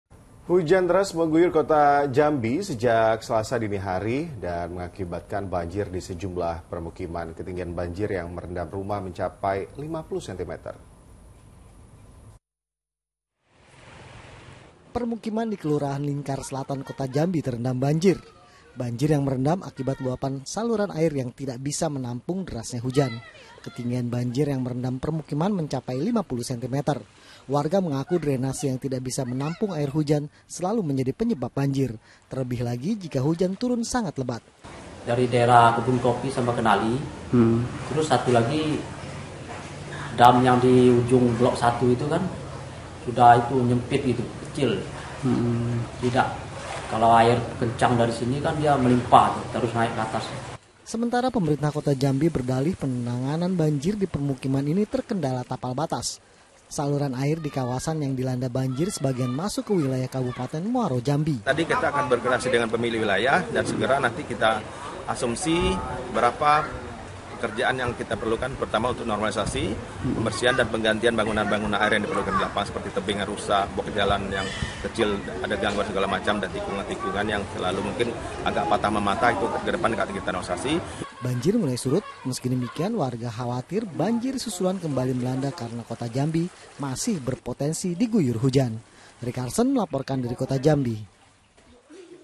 Banjir Rendam Permukiman di Jambi Setinggi 50 Cm | Liputan 6
Hujan deras mengguyur kota Jambi sejak Selasa (23/5) dini hari mengakibatkan banjir di sejumlah permukiman. Ketinggian banjir yang merendam rumah mencapai 50 centimeter.